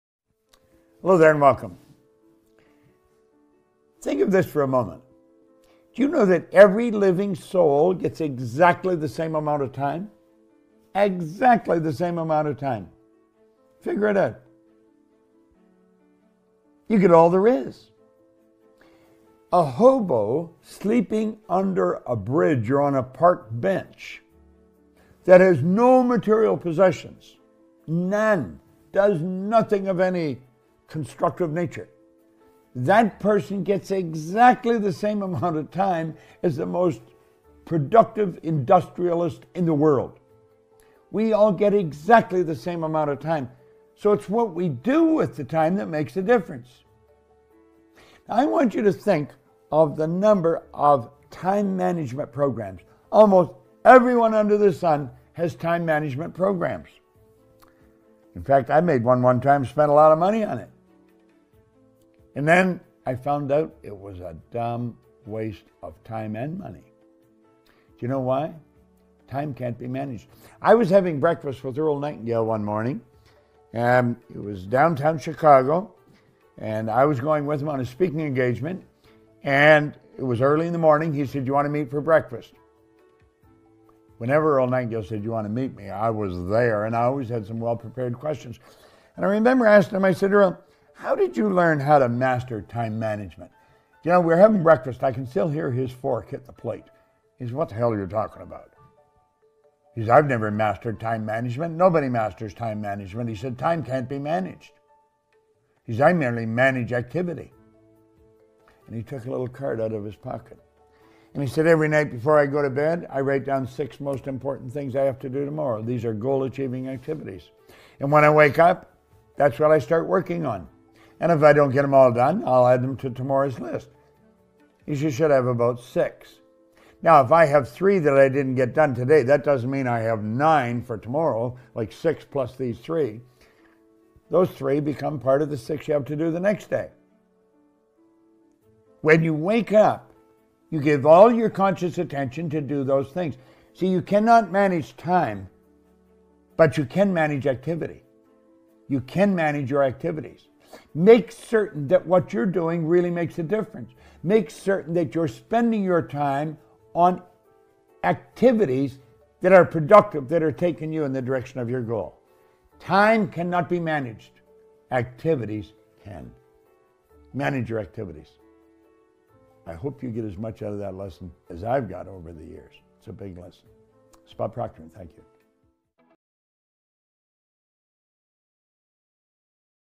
Bob proctor|Apa kita membutuhkan manejemen waktu. | Subtitle Indonesia
Kita tidak bisa menguasai waktu
Yang kita bisa adalah memanfaatkannya
Produktivitas dengan cara membuat daftar
Aktivitas setiap harinya